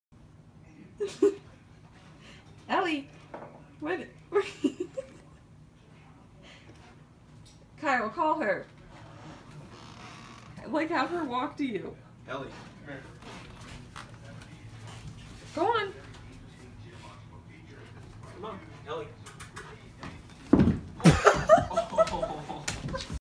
Puppy in Cow Blanket
I put my cow blanket on my dog and she was adorable, but also had a hard time walking.